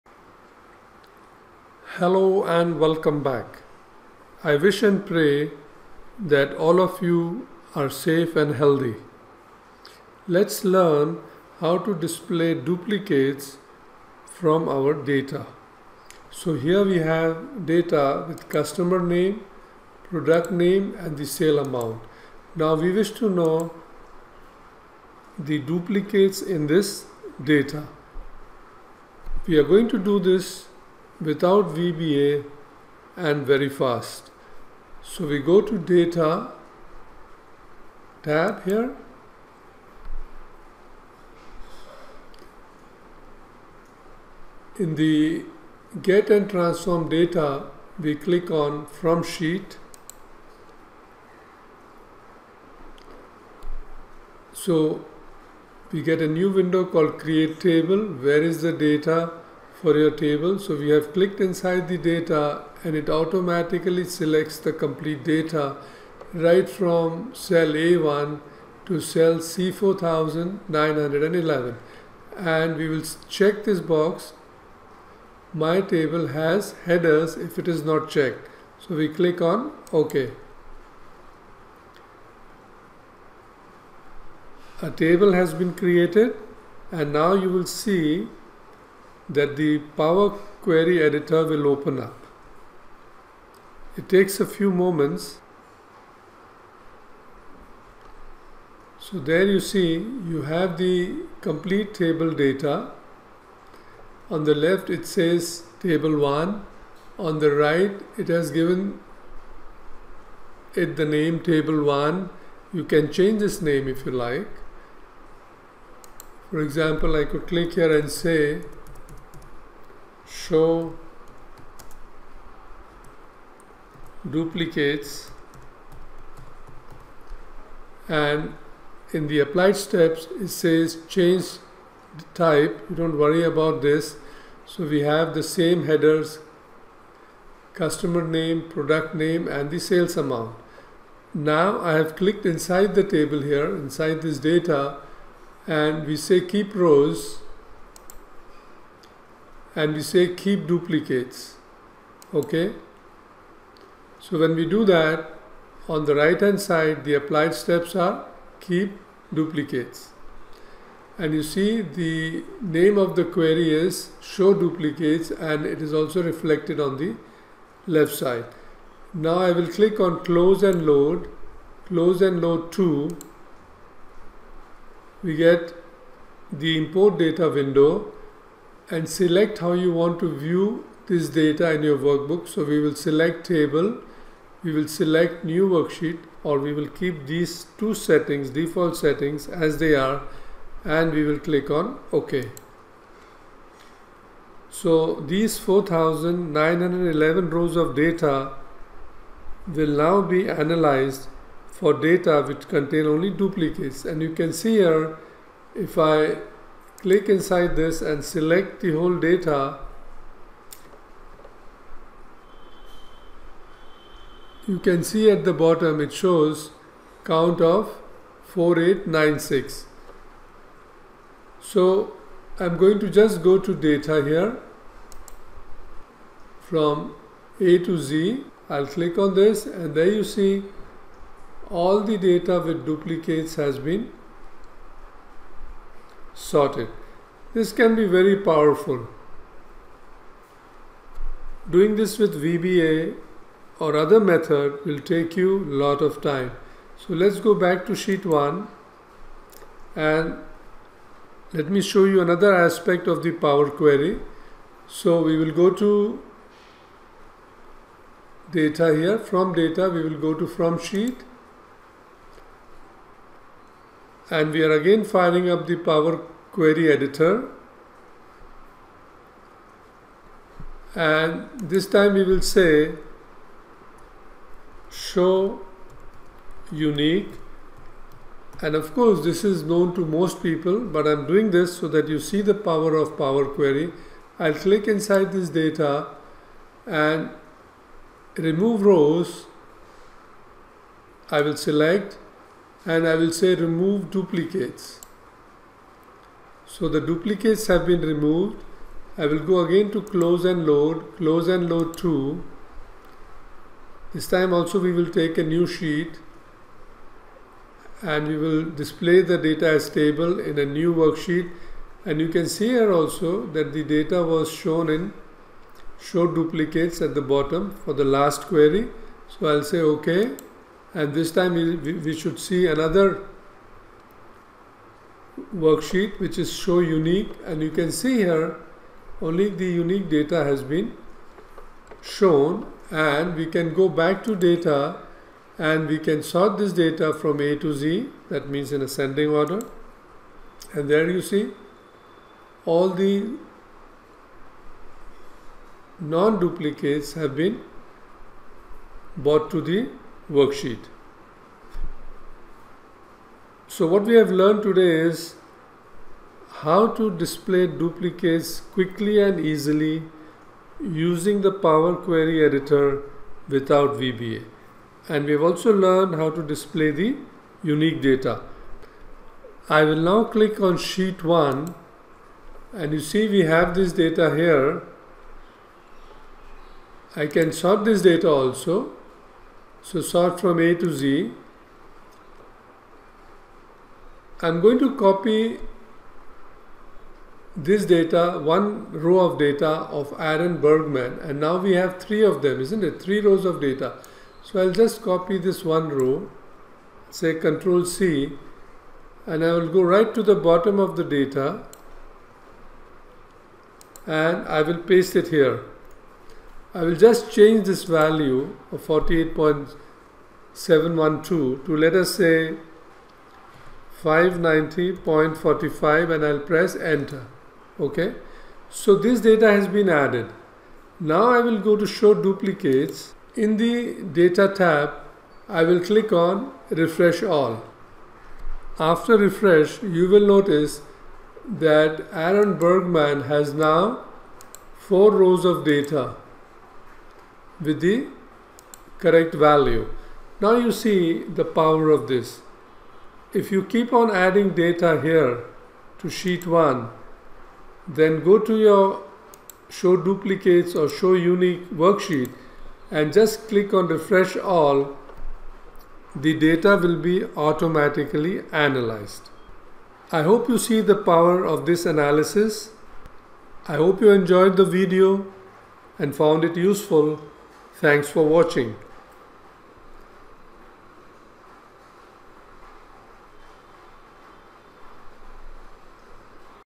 Display Duplicates or Unique Values Using Power Query | 2021
How to display duplicates or unique Values using Power Query.